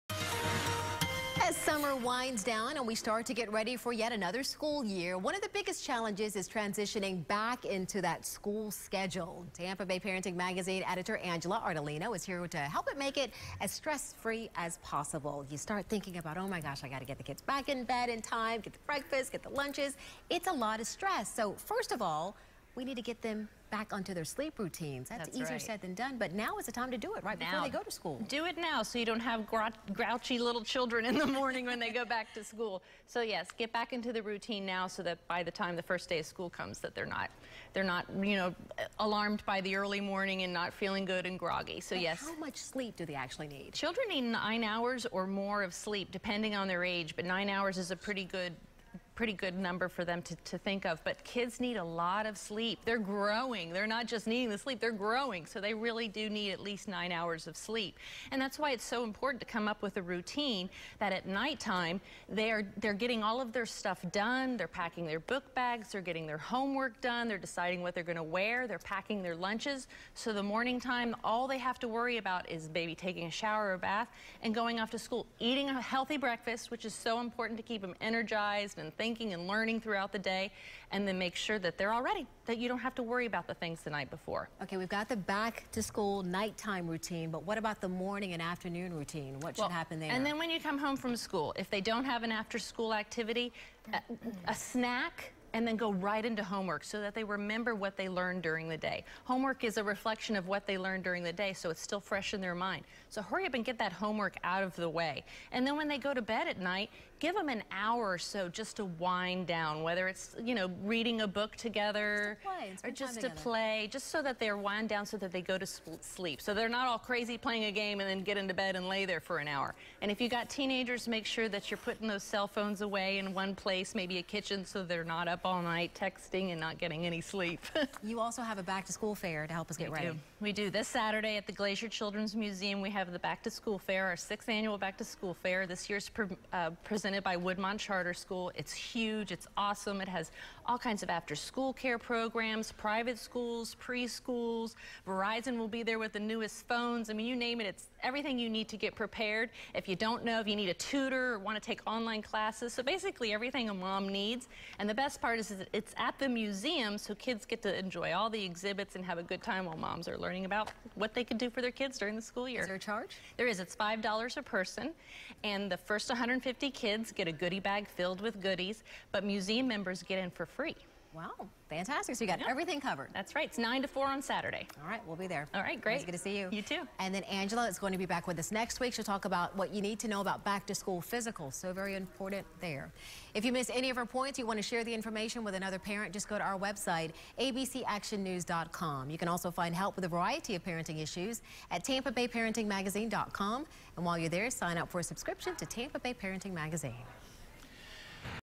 Parents in Action: School week routine
The key to a stress free transition into a school time routine is starting early. Here are some things you can do now to kids ready.